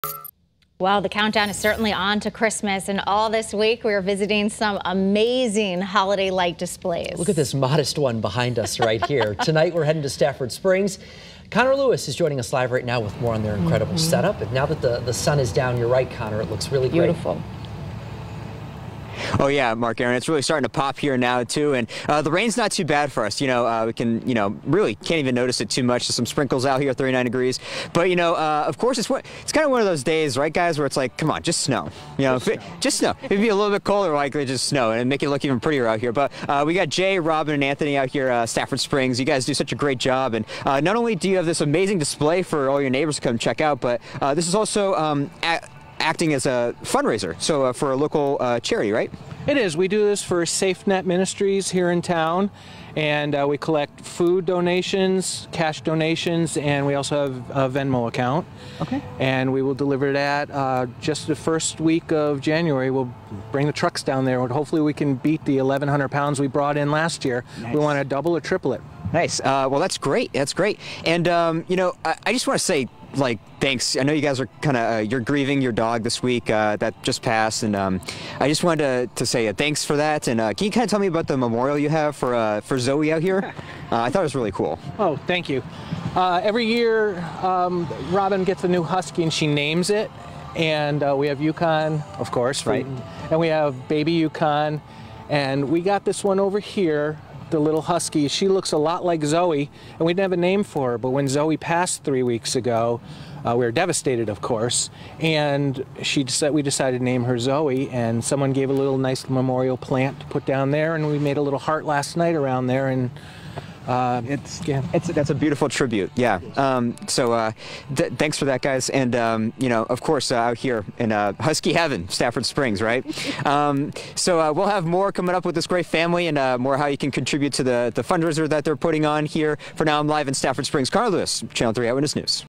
HOLIDAY LIGHTS: Stafford Springs home spreading holiday cheer
We're touring the state and highlighting different light displays.